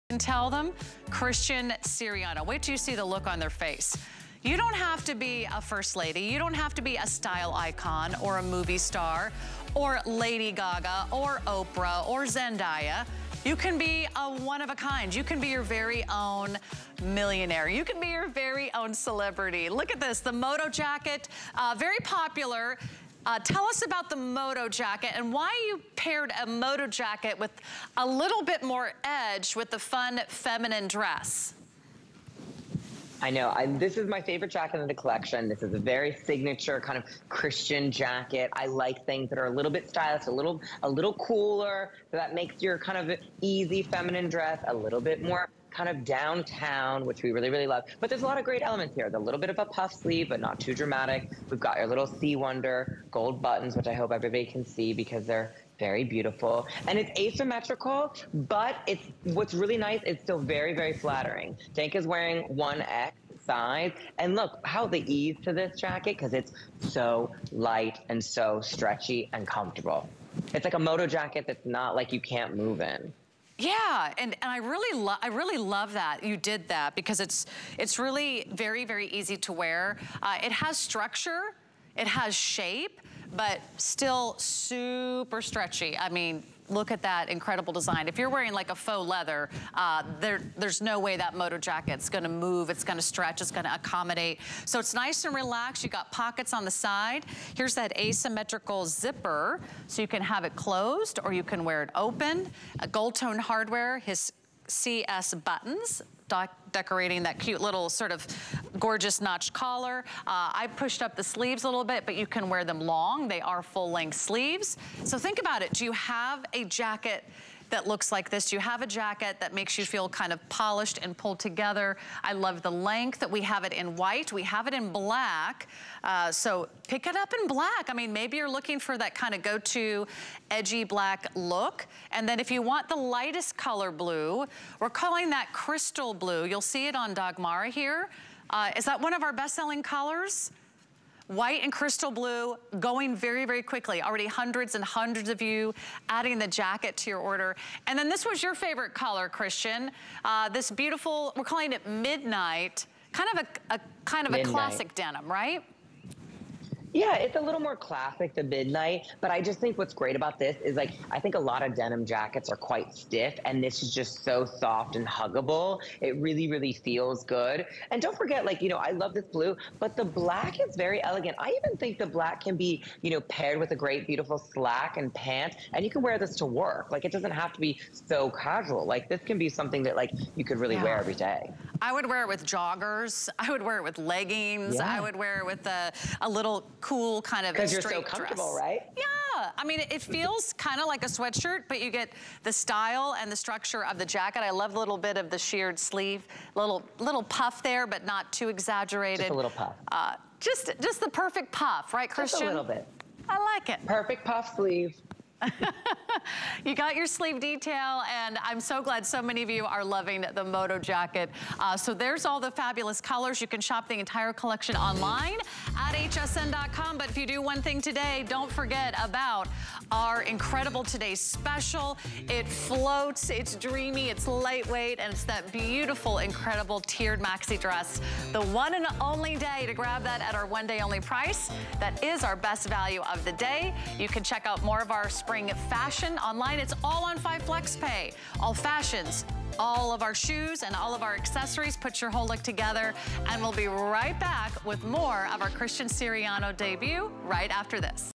C Wonder by Christian Siriano Puff Sleeve Moto Jacket
Live life on the edge. This Wonder Stretch Twill, puff sleeve jacket adds a cool element to your workdaytoweekend looks.